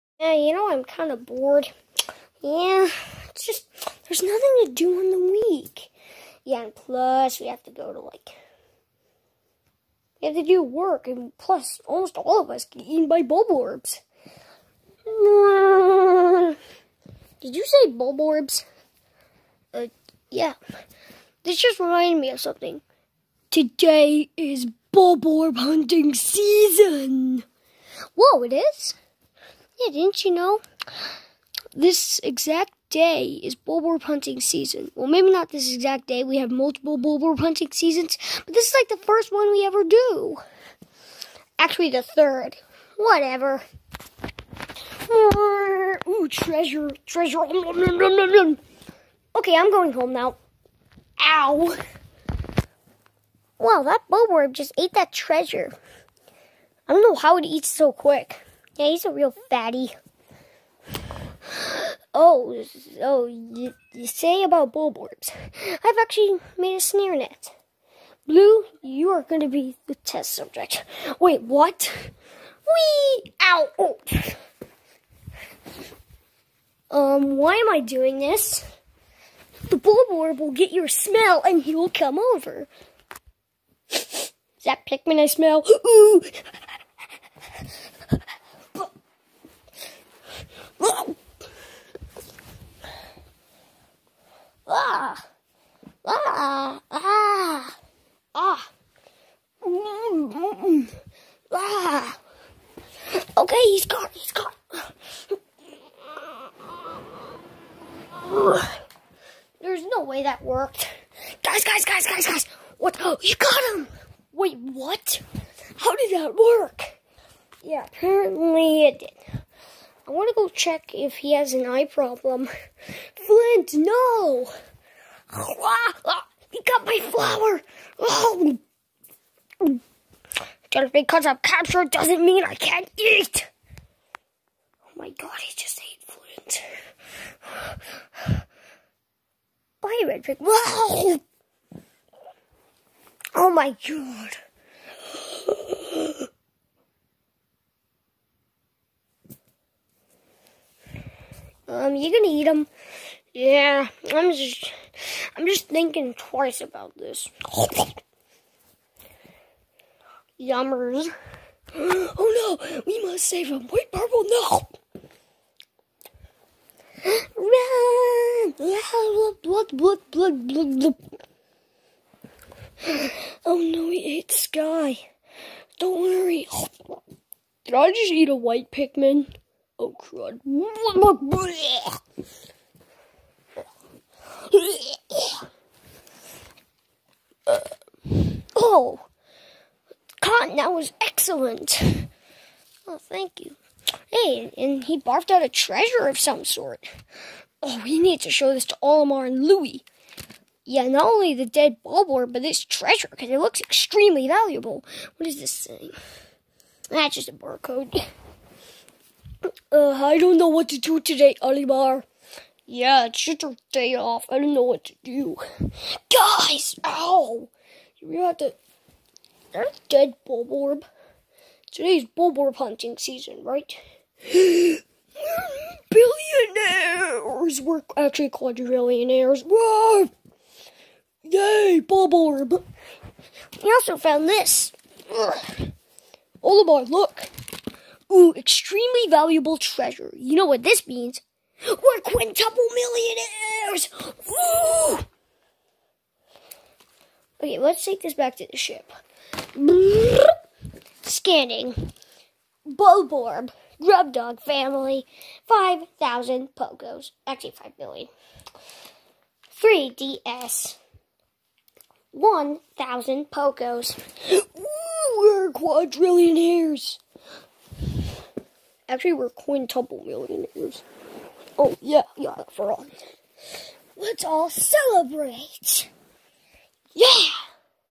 Pikmin's Quest Episode 1: Bulborb Hunting Season
Pikmin's Quest Is a Family Friendly, Wacky Show About Pikmin, The Pikmin Are On A Quest to Defeat The Lengenday Bulbking, This Episode Is About The Pikmin Catching And Selling The Bulborb And The Treasure He Ate

Characters:
Olimar
Louie
Flame [Red]
Aqua [Blue
Sunshine [Yellow]
Flint [Rock]
Plum [Purple]
Sky [Winged]
Cotton [White]
Bulborb